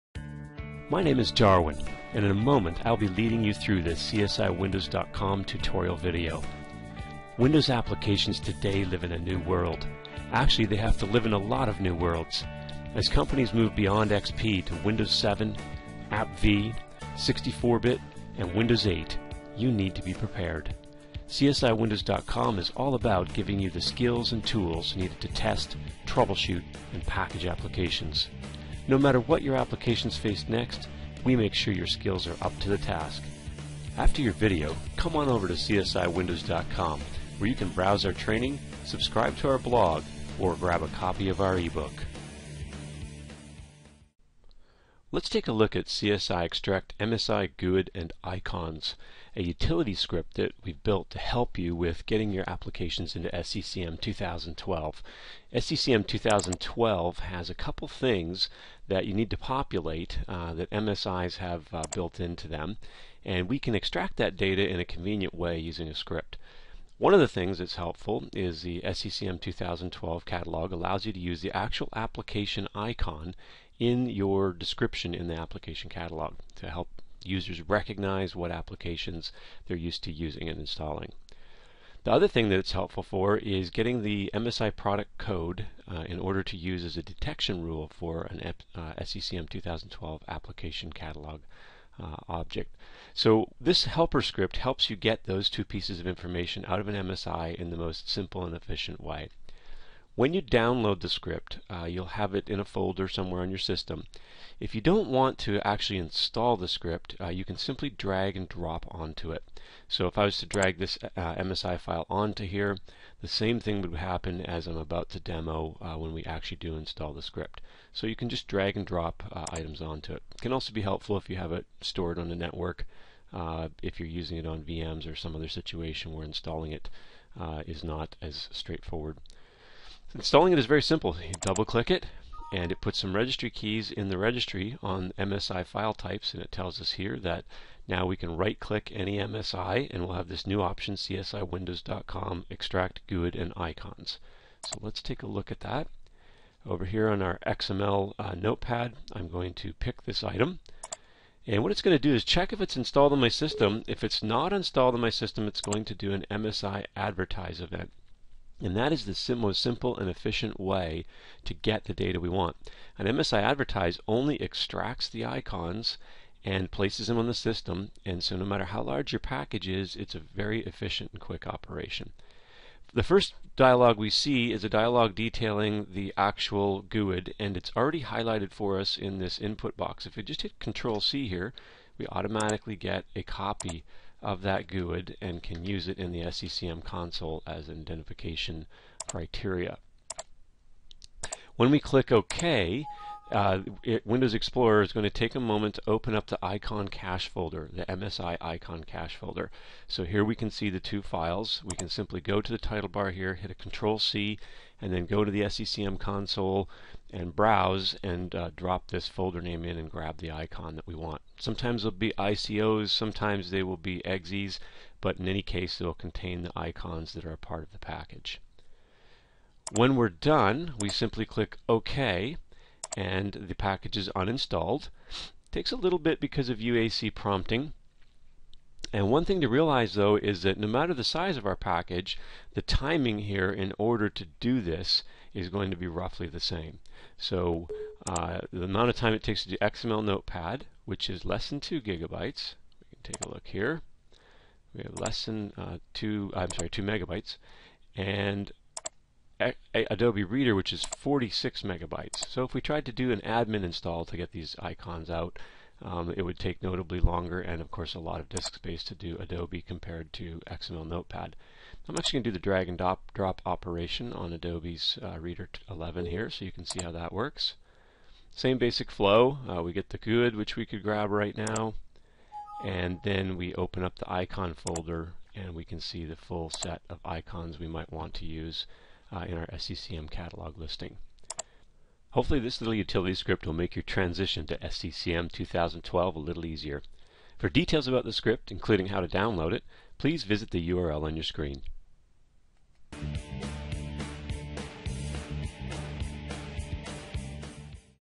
CSI-Windows.com: Helper Script from Extracting MSI Icon and Product Code For SCCM 2012
This helpful utility script allows you to very quickly extract Icons and Product Code for SCCM 2012 Application Catalog.  It works very quickly even on very large packages.